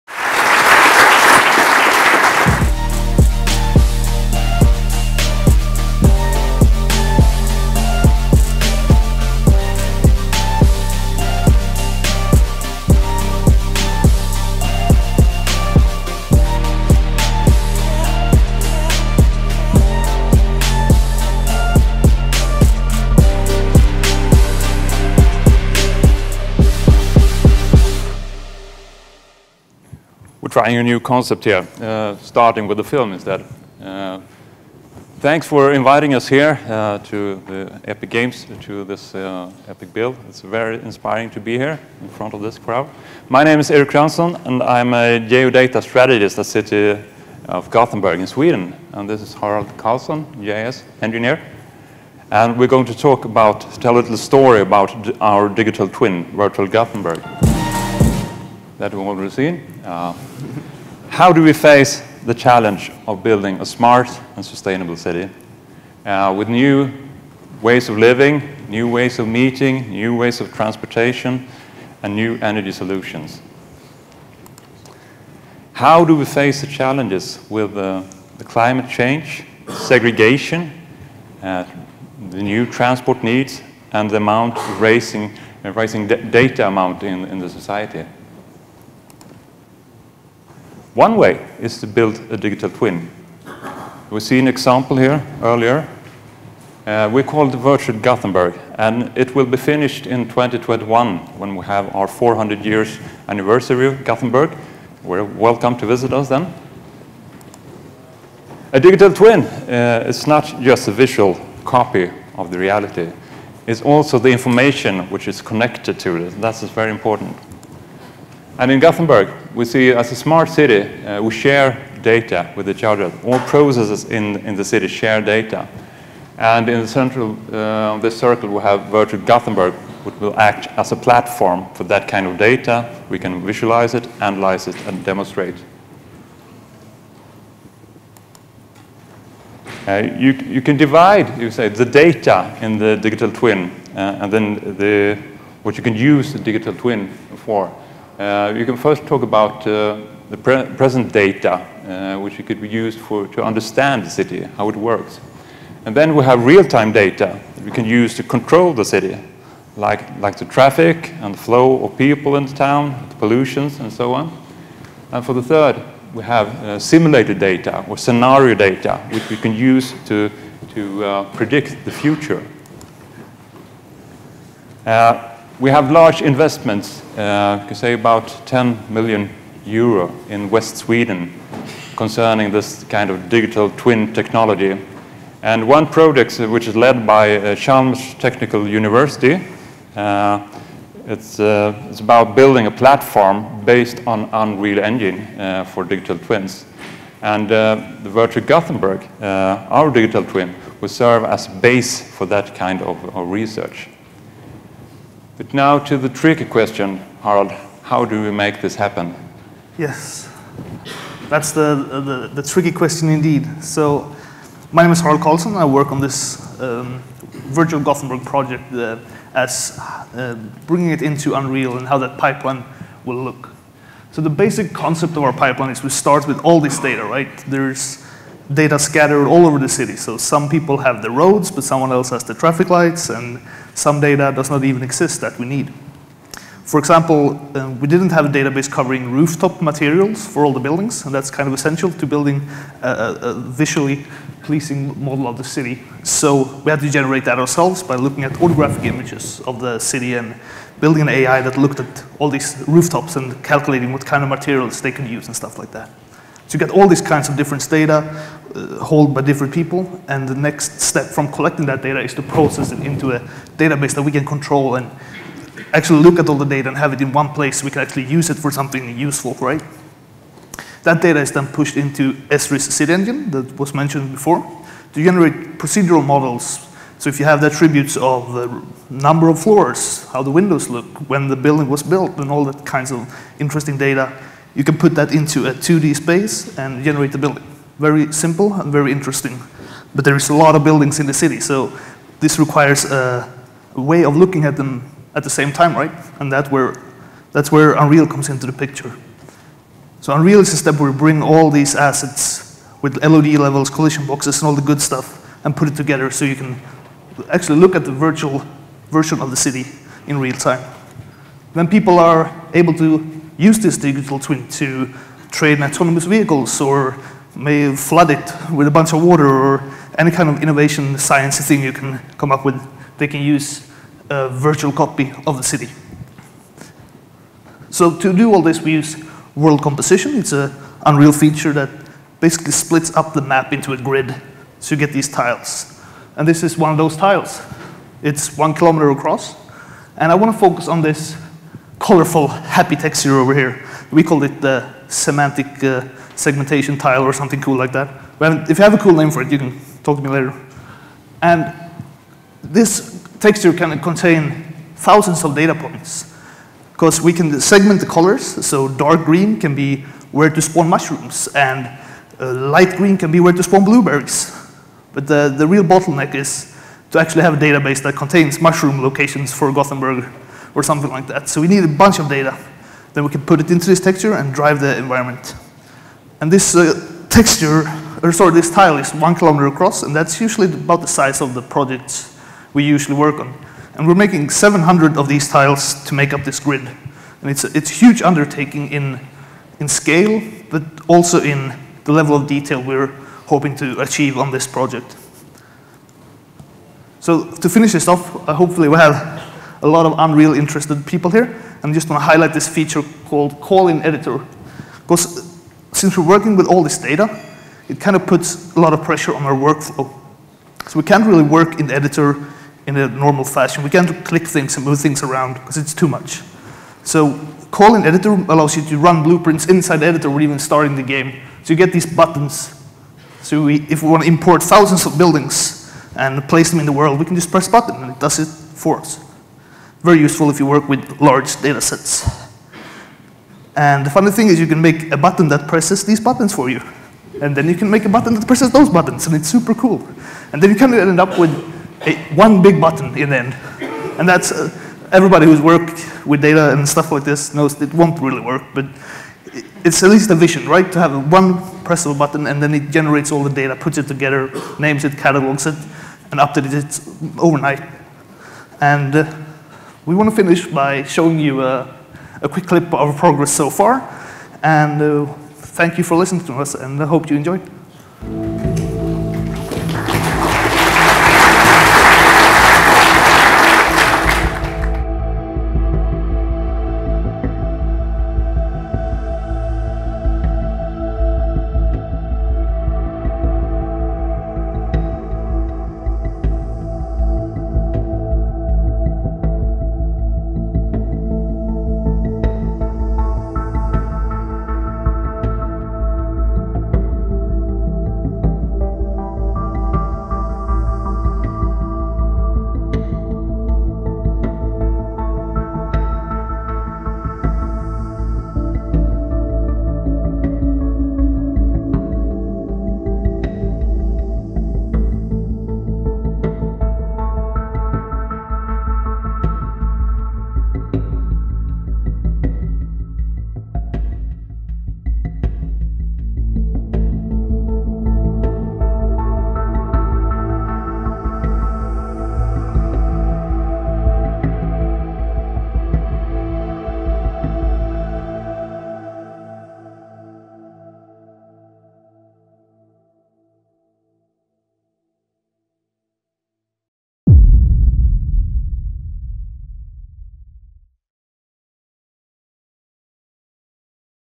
Virtual Gothenburg | Build: London '19 for Architecture | Unreal Engine
Find out how Swedish Geostrategist Eric Jeansson and GIS-Engineer Harald Karlsson are using real-time technology to predict and plan for the future needs of Gothenburg City and its residents.